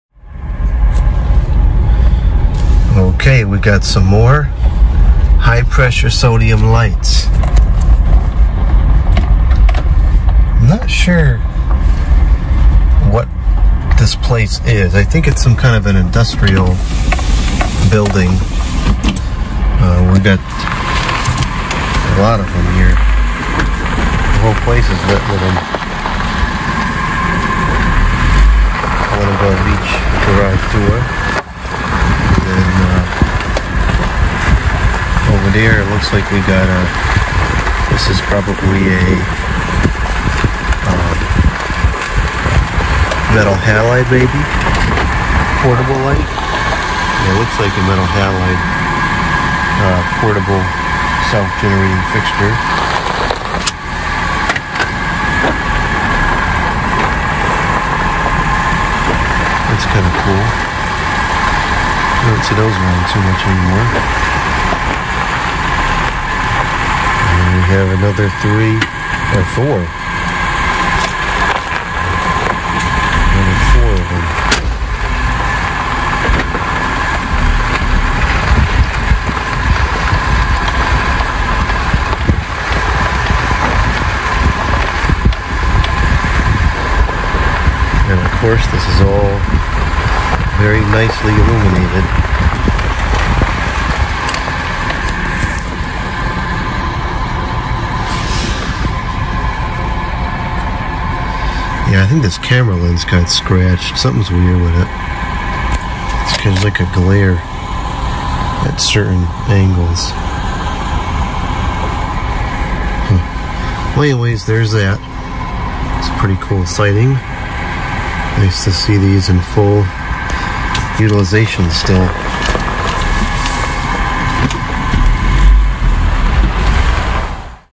High Pressure Sodium Flood Lights and a Metal Halide Portable Fixture
Some very high power HPS (high pressure sodium) light fixtures in some kind of industrial area, as well as a portable self-generating metal halide construction light.